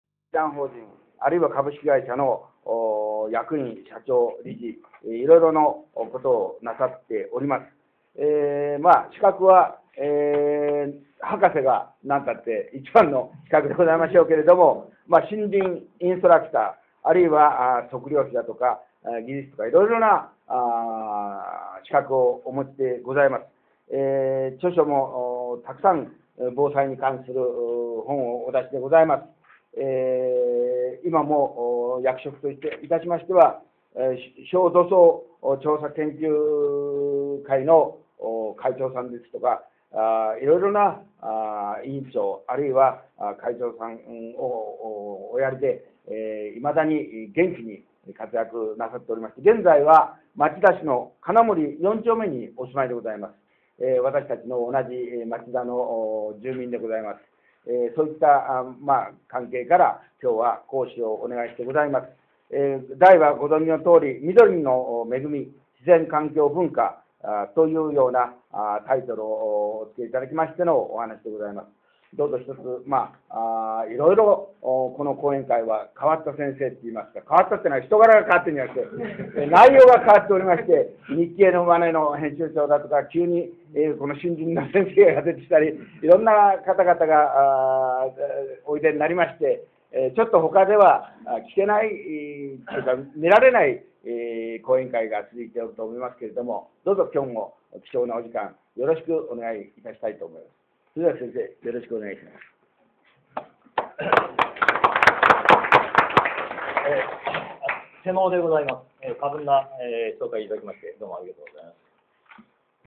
セミナー講師紹介 瀬尾克美先生紹介
09.03.21吉田つとむ後援会 第14回セミナー講師紹介　　森林インストラクターの瀬尾克美先生紹介　テーマは、「みどりの恵み（ 自然、文化、環境）」